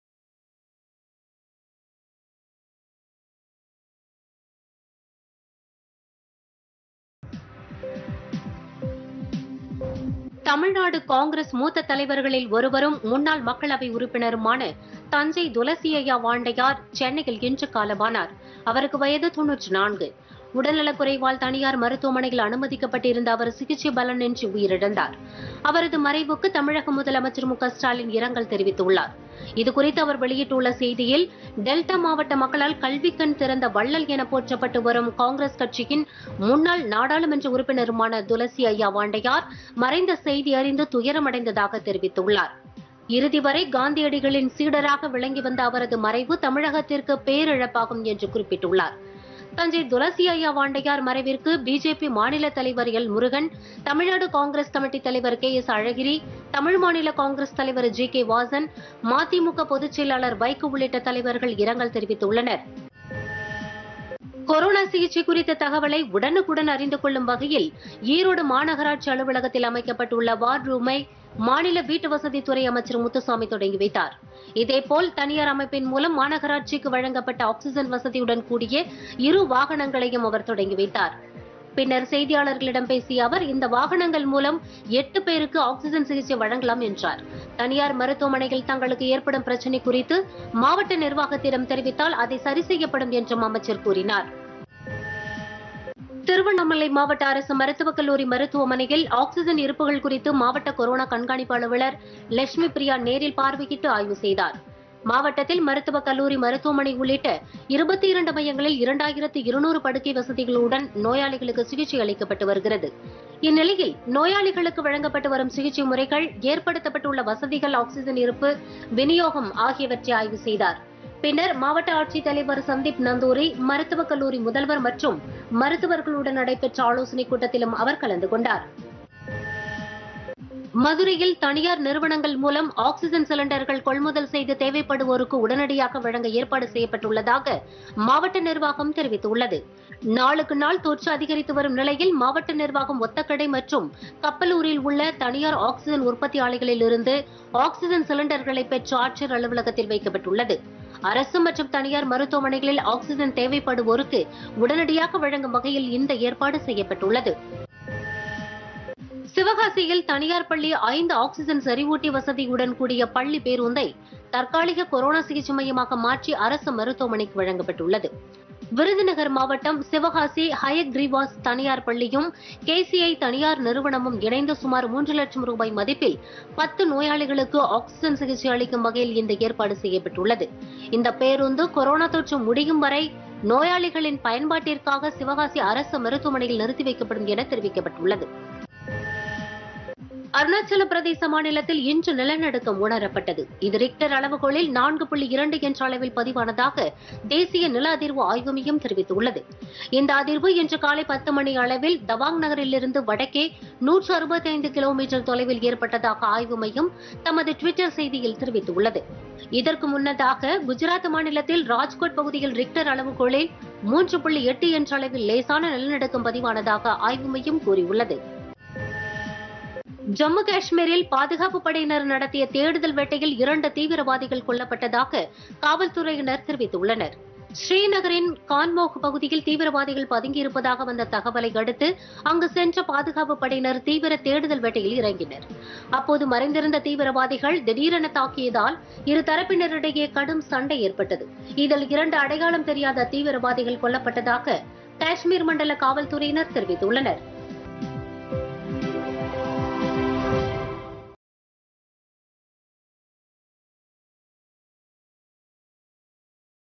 பொதிகை பிற்பகல் 2.00 மணி விரைவுச் செய்திகள் [17.05.2021]
1)   முதலமைச்சர் இரங்கல்

2) ஈரோடு - வார் ரூமை திறப்பு

3) ஆக்ஸிஜன் இருப்பு குறித்து ஆய்வு

4)  தனியார் ஆலையில் ஆக்ஸிஜன் கொள்முதல்

5) ஆக்ஸிஜன் செறிவூட்டி வசதியுடன் பேருந்து

6) அருணாசலப்பிரதேசத்தில் நிலநடுக்கம்

7) ஜம்மு-காஷ்மீர்: 2 தீவிரவாதிகள் பலி